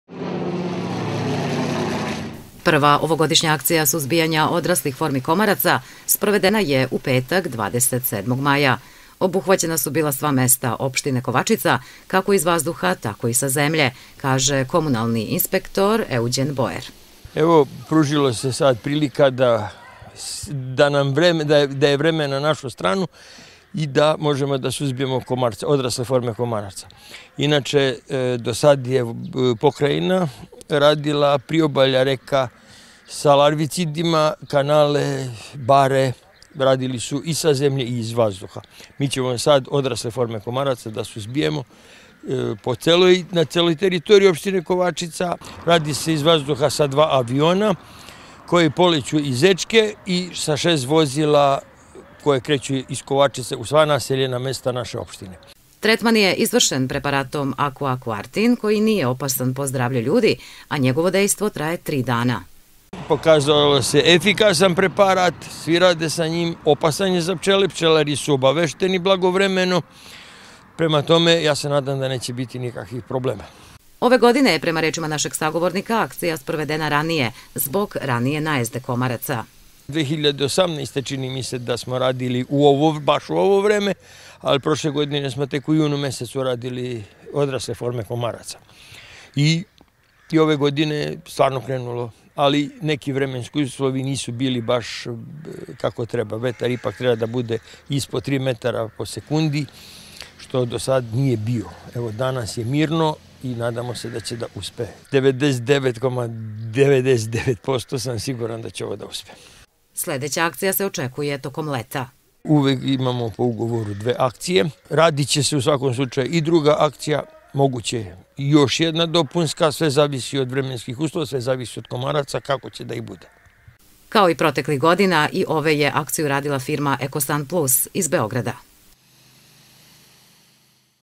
Sprovedena akcija suzbijanja komaraca na teritoriji opštine Kovačica
Akcija suzbijanja odraslih formi komaraca na teritorije opštine Kovačica sprovedena je u petak 27. maja, kako iz vazduha tako i sa zemlje.